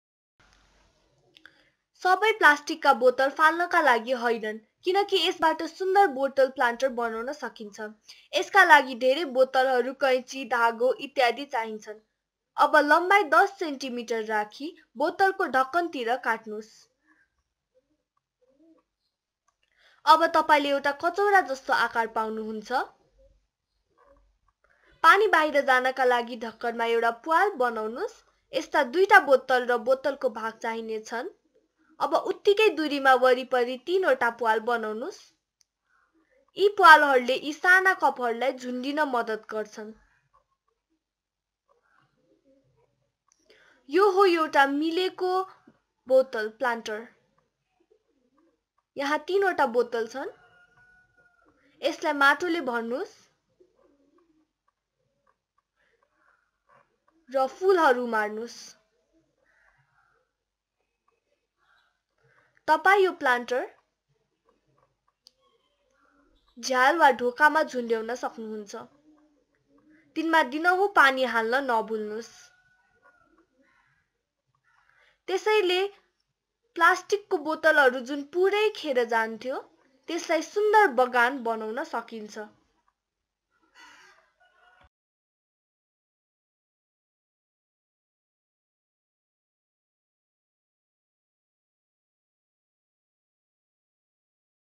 Bottle Planter | Nepali
Empty plastic water bottles have become a great pollution menace. But you could recycle them to make beautiful BOTTLE PLANTERS. Cut 1-cm of the bottle from the top end. It will form a bowl. Make a hole in the lid for the water to drain out. You will need two such bowls and one base from a bottle. Make three holes in the base bowl so as to hang it by a strong string. Finally hang all the three bottle bowls as shown. Fill these bowls with soil and sow beautiful plants and flowers. Do not forget to water them regularly. You can beautify your window sill with these wonderful recycled bottle planters.

Dubbed in Nepali by ThinkInk (thinkinksite.com), Kehi Garaun (kgnepal.com.np)